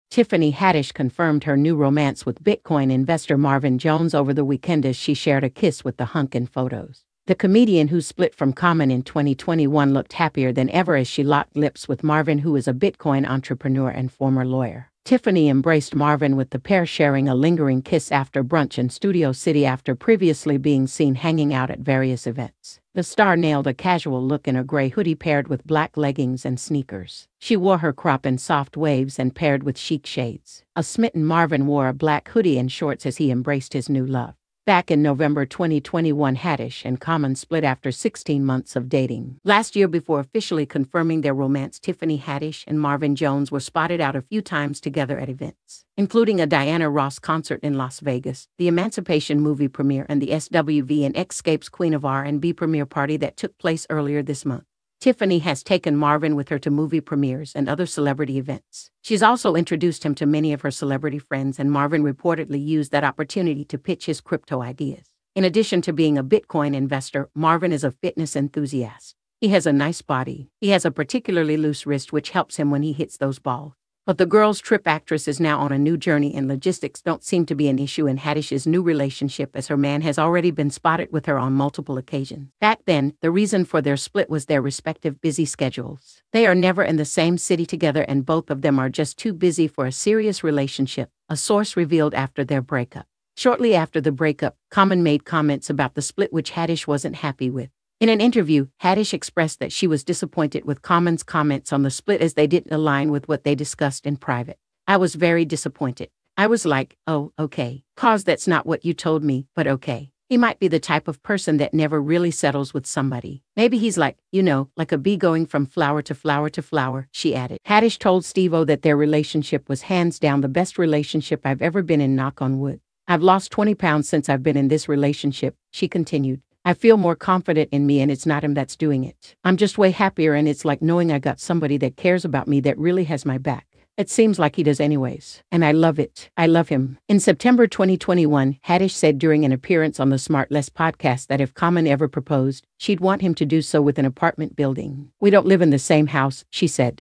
Tiffany Haddish Confirms New Romance With Marvin Jones as Pair Share a Sweet Kiss After Brunch in LA
Tiffany Haddish confirmed her new romance with Bitcoin investor Marvin Jones over the weekend as she shared a kiss with the hunk in photos. The comedian, who split from Common in 2021 looked happier than ever as she locked lips with Marvin who is a Bitcoin entrepreneur. Tiffany embraced Marvin with the pair sharing a lingering kiss after brunch in Studio City after previously being seen hanging out at various events. The star nailed a casual look in a grey hoodie paired with black leggings and sneakers. She wore her crop in soft waves and paired with chic shades. A smitten Marvin wore a black hoodie and shorts as he embraced his new love. Tiffany Haddish and Marvin Jones were spotted out a few times together at events including a Diana Ross concert in Las Vegas, the “Emancipation” movie premiere and the SWV and Xscape’s “Queen of R&B” premiere party that took place earlier this month.